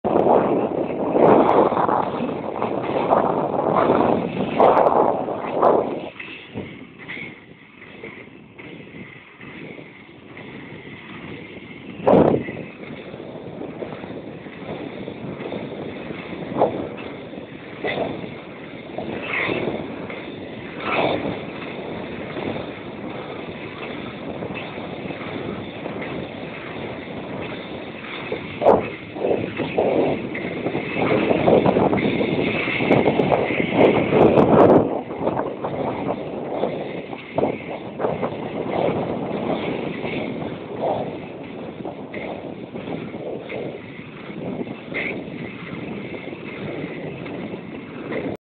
Rychlobruslení
jízda po cyklostezce z Hluboké n/Vlt. v Českých Budějovicích